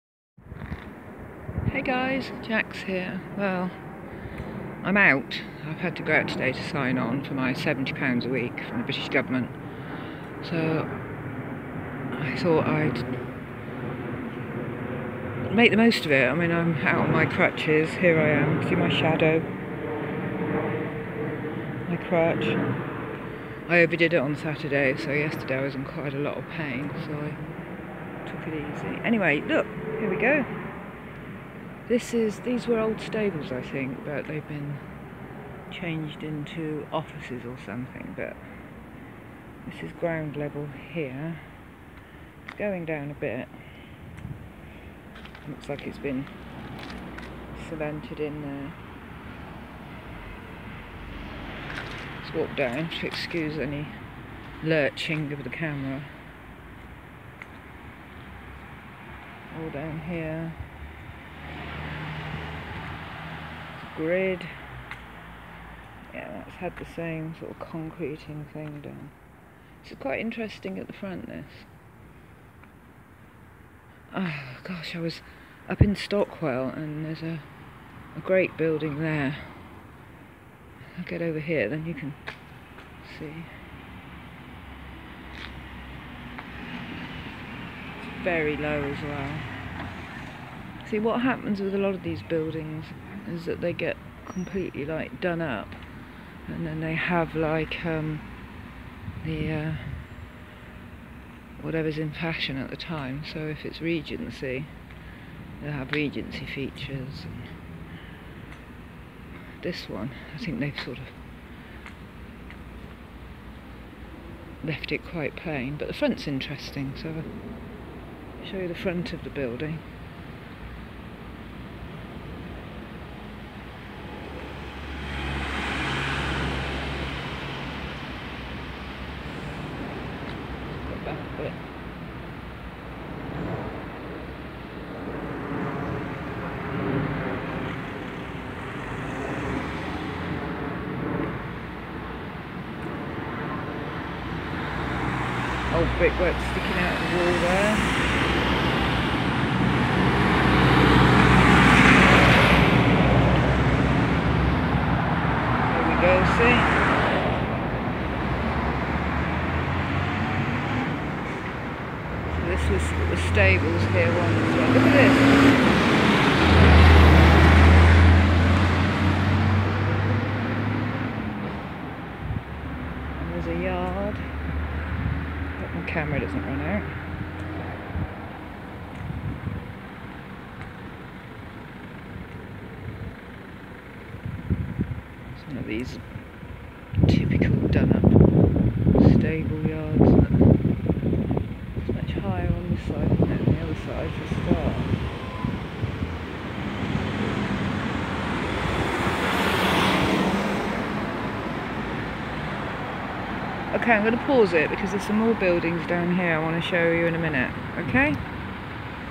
Mud flooded old 'stables' Wandsworth... London
The inner courtyard is un mudflooded... it seems that if there was an enclosed inner area it escaped the mud flooding which channelled down the streets... like water down gulleys.